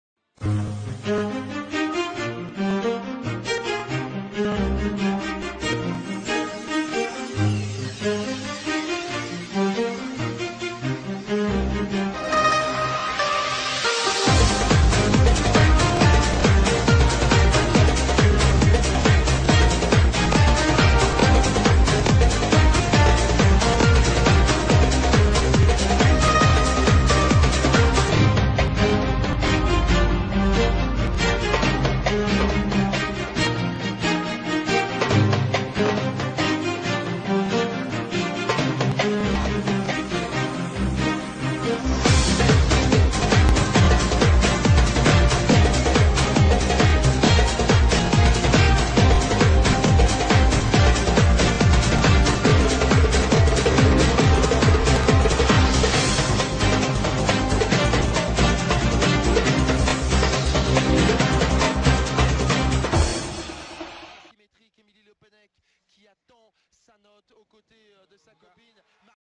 Dream Bar Routine
This is a new video about the bar routine that I consider as my "dream".
I took 3 big skills did on the uneven bars by different gymnasts.
I have no idea of the code of the points for the ub, so if something is missing in the routine, please comment to tell me and I'll try to add it.
Starring : Rebecca Bross - Aliya Mustafina - Tatiana Nabieva - Emilie Le Pennec - Victoria Komova.